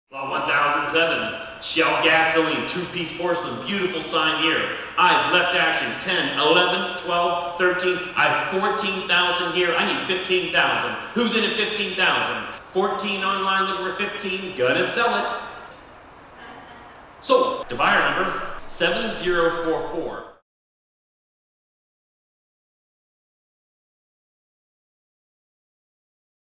Selling an Outstanding Shell Two-Piece Porcelain Neon Sign
Wow! What an outstanding piece of Shell Gasoline Advertising. This is one of the best examples of this sign we have ever sold. Two Single Sided Porcelain Signs mounted back to back, with beautiful glowing neon along the outer edge. Each sign is in outstanding condition, showing excellent color & gloss, with almost no wear whatsoever. The outer edges are extremely clean, and the piece still retains all four of its original mounting brackets. Side one shows outstanding color & gloss, with no notable wear other than some very light chips at the mounting bracket holes. Side two is also in incredible condition, showing excellent color & gloss, with one pencil eraser size chip in the E of Shell, and one very small chip in the lower flutes below the lettering. Side two also shows some very minimal chipping near the bracket mounting holes. Overall an outstanding piece of Shell Advertising that would be very hard to upgrade.

This sign sold for $17,220.